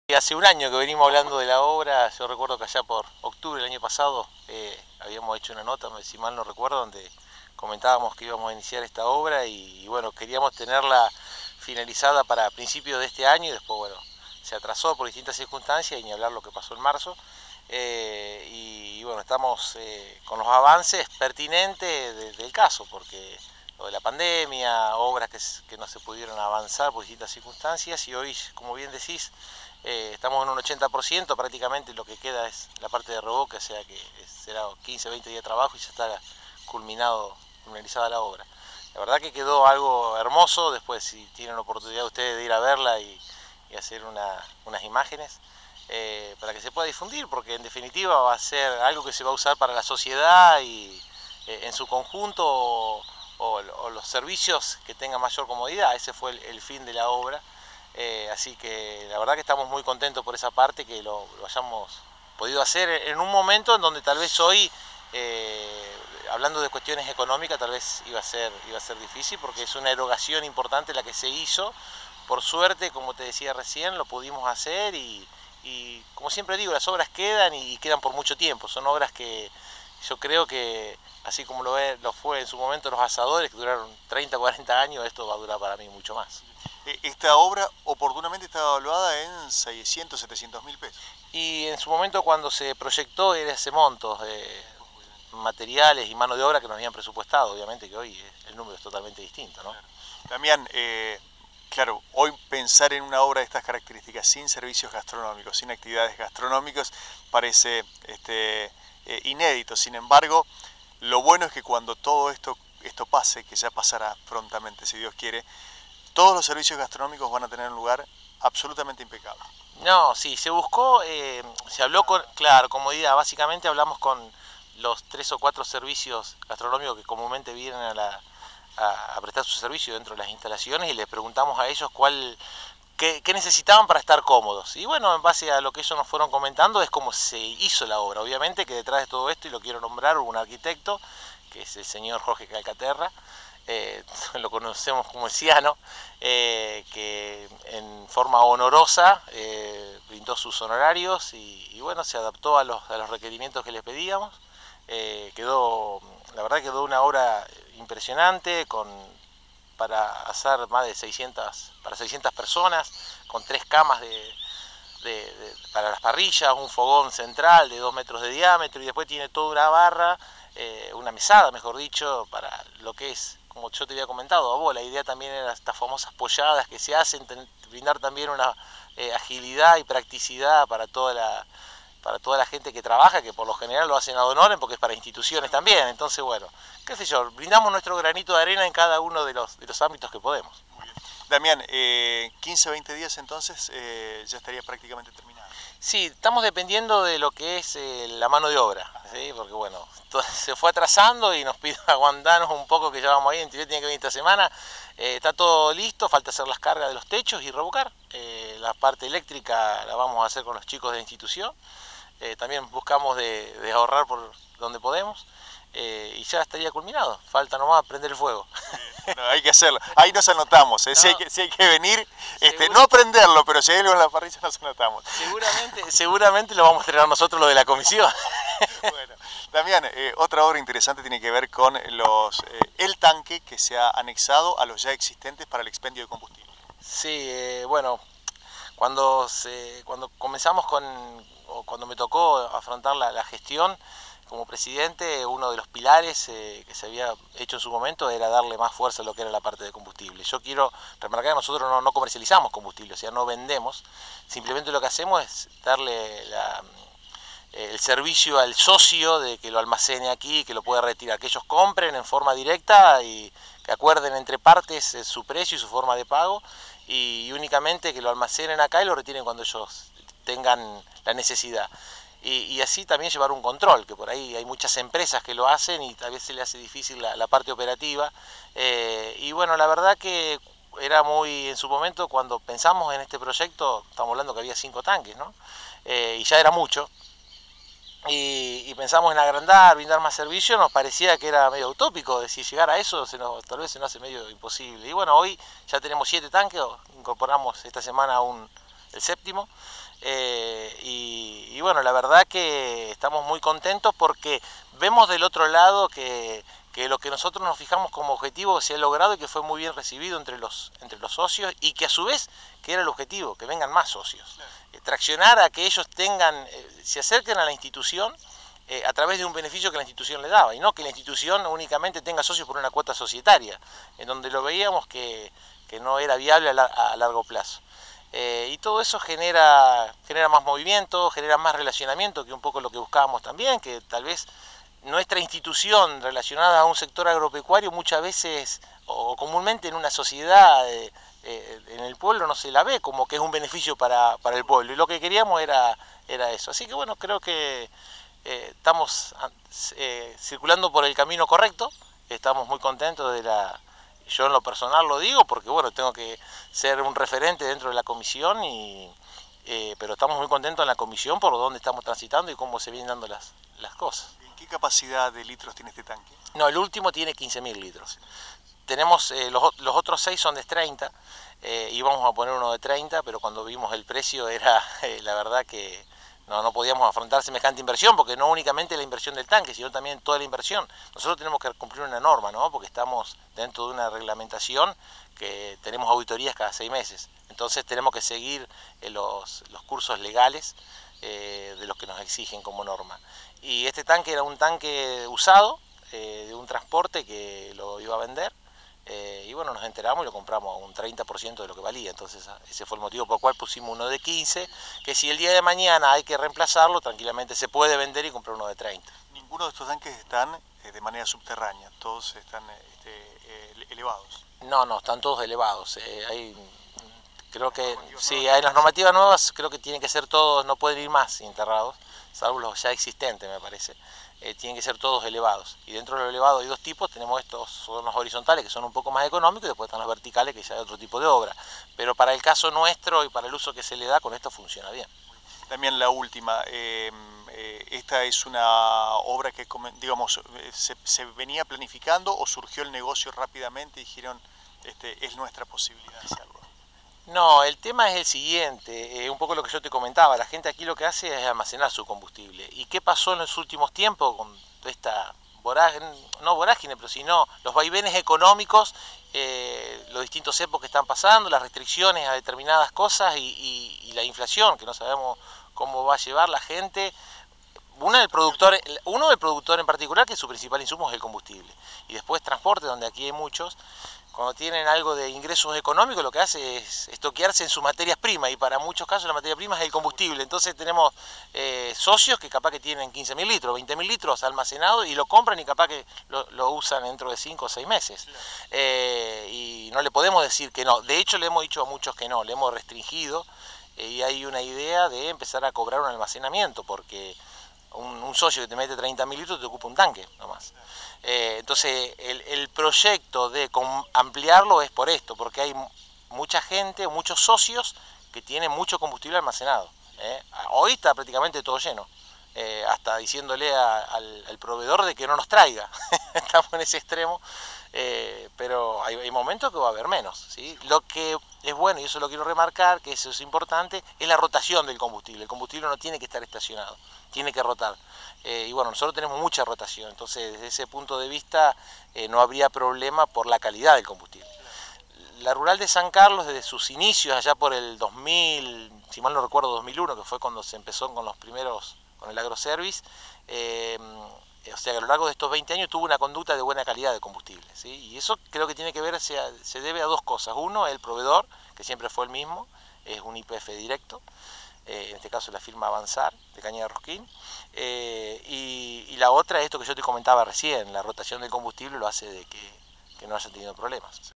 Damián Pafundi - Pte. Sociedad Rural de San Carlos
La obra de nuevos parrilleros está a punto de culminar